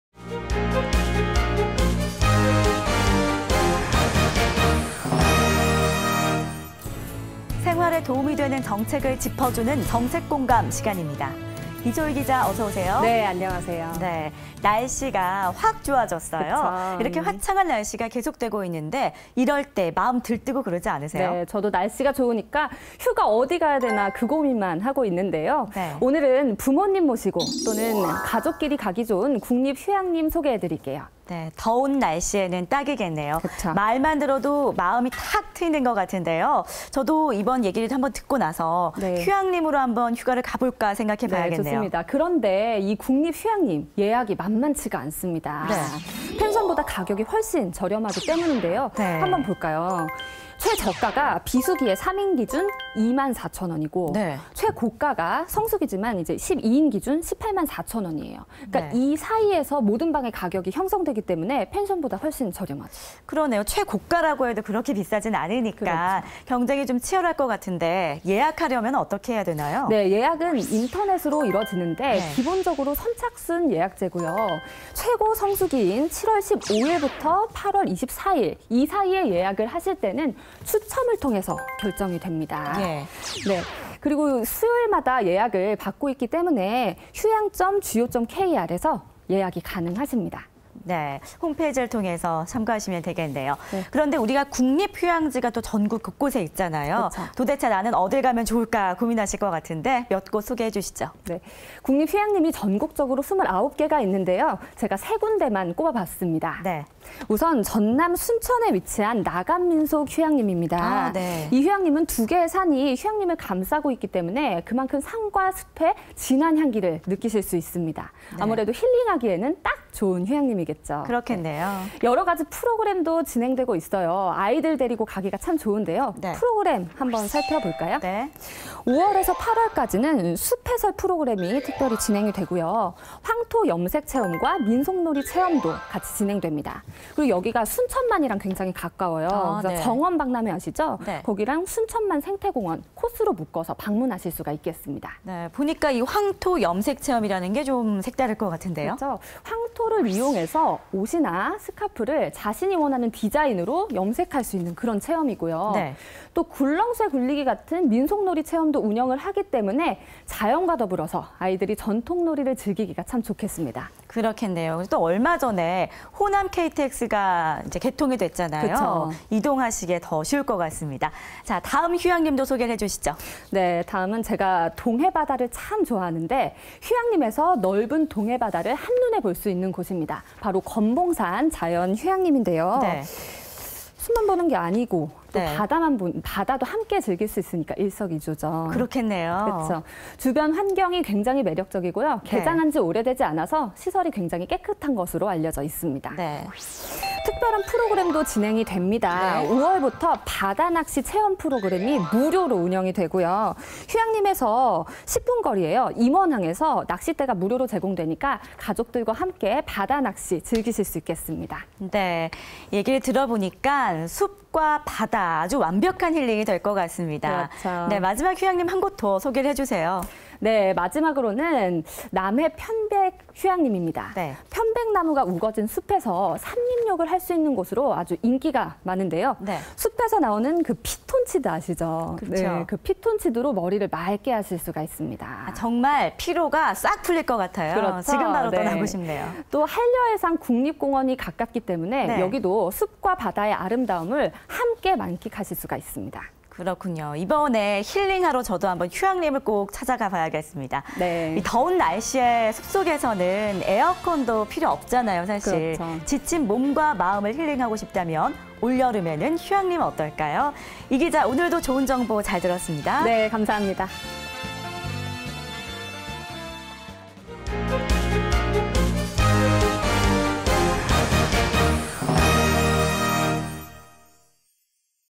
가족과 함께 힐링하기 좋은 곳! '국립휴양림' - 정책 공감 -
빽빽한 숲 속에서 삼림욕 해보셨나요?
힐링 휴가 원하신다면 국립휴양림 추천해드립니다!
펜션보다 싼 가격에 아름다운 휴양림을 누리고 싶다면 목요일 11시 '정책공감'을 확인하세요~!